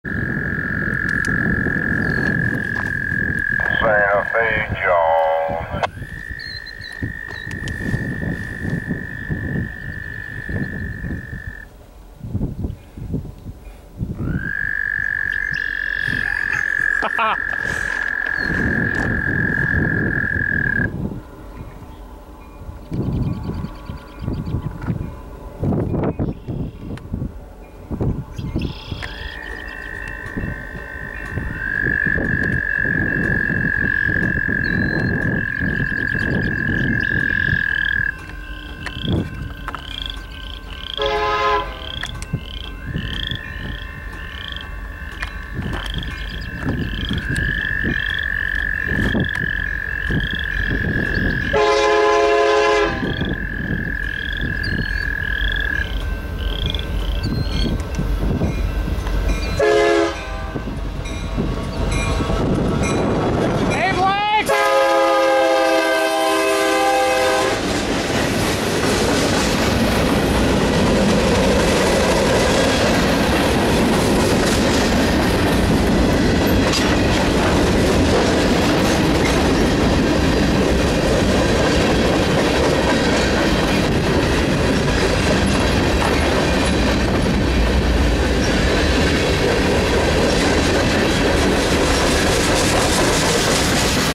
Green Machines!! Awesome Show from BNSF 8114
Blake was in a good Mood this day...
He called me on the radio, flashed the headlights (pardon my laughter), and just put on an overall great show!